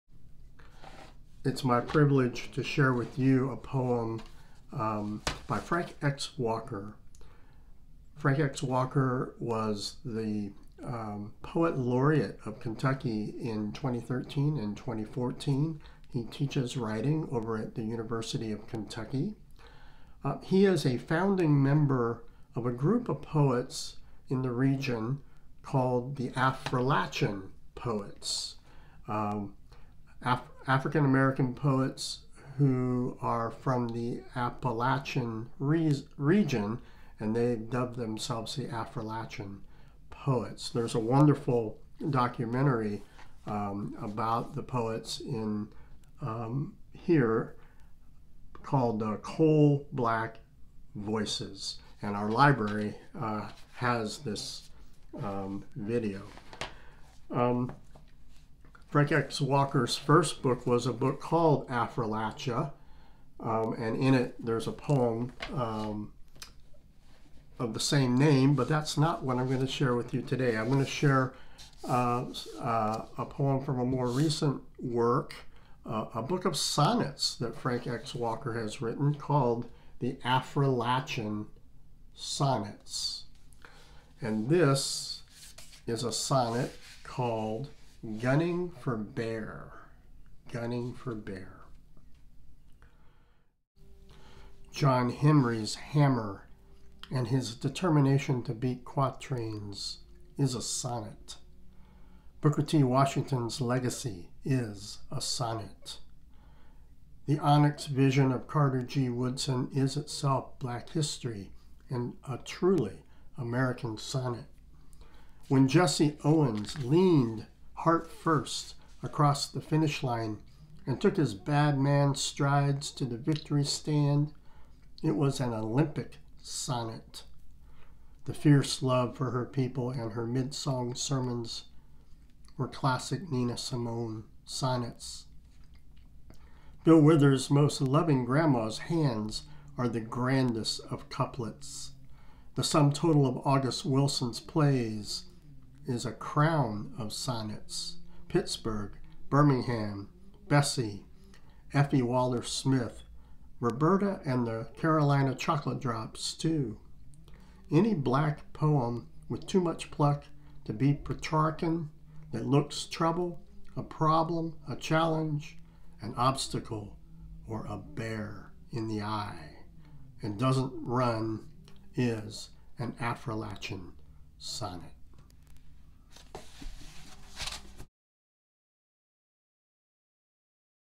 "Gunning for Bear" by Frank X. Walker
Read by Louisville Seminary professor Brad Wigger for the African American Read-In